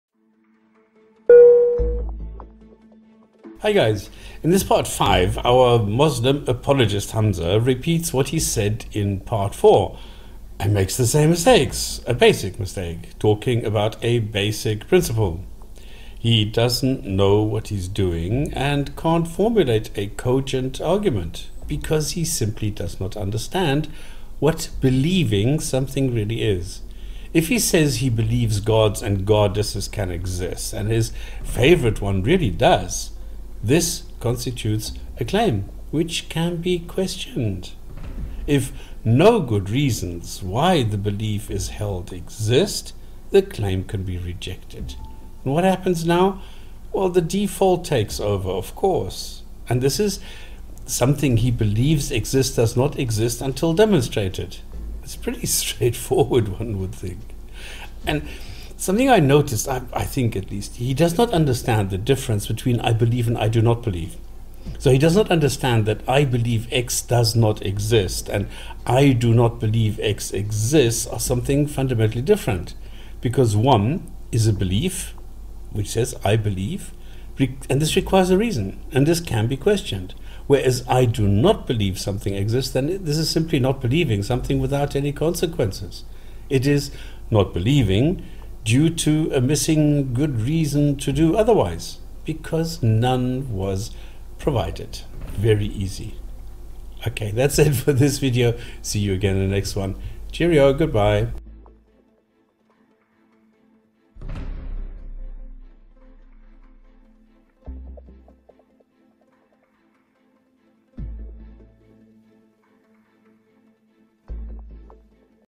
Case Study - Muslim Apologist Hamza – 5 Repeat
This is a refutation video of the mini-series on Hamza, the Muslim apologist of his video number 5. He repeats the mistakes made in basic principles and again fails. Badly.
It shows me once again, that a) there are no good reasons to believe gods and goddesses exist and b) an a-theist can understand and put themselves into the shoes of a theist – but not the other way around.
As well as addressing the claims and showing what reality shows, I offer criticism of the way the Koran is presented by Muslims to other Muslims and non-Muslims alike.